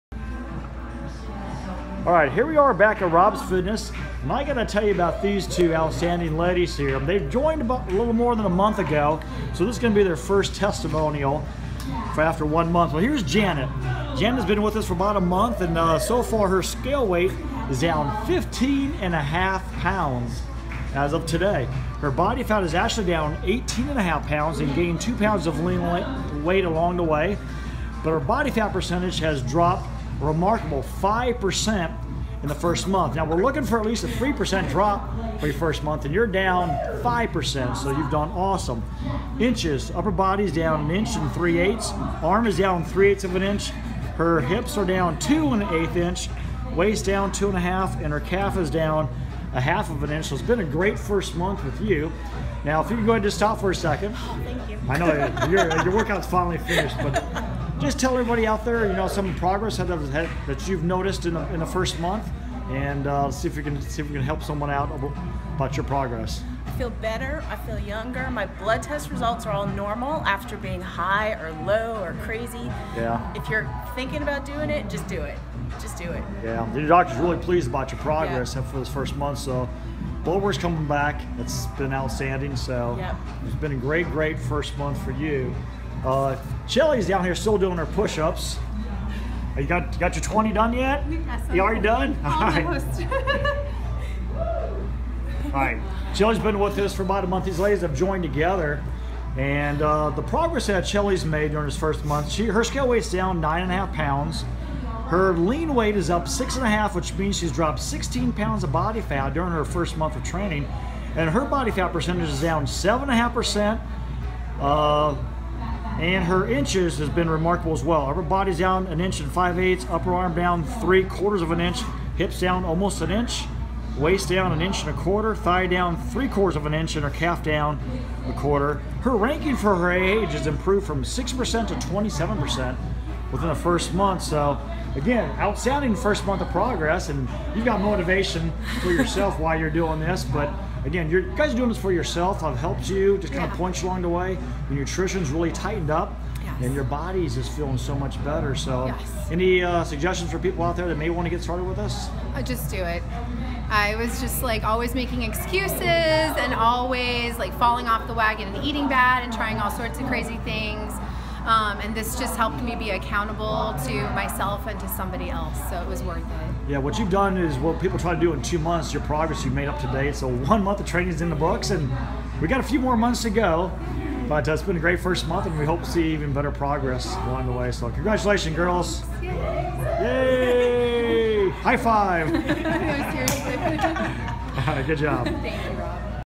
Janet & Chely - 1st month video testimonial
Janet and Chely have been working at RobZFitness for the past month. Their testimonials blew me away...

Janet is down 18.5lbs of body fat and has increased her lean weight by 2.5lbs. So overall her  scale weight is down 15.5lbs and overall dropped 5% body fat during her first month.

Chely is down 16.5lbs of body fat and has increased her lean weight by 6.5lbs. So overall her  scale weight is down 9.5lbs and overall dropped 7.5% body fat during her first month.

If you want results like Janet and Chely schedule your FREE fitness consultation with me at or drop me an email at and let's see if my program is right for you.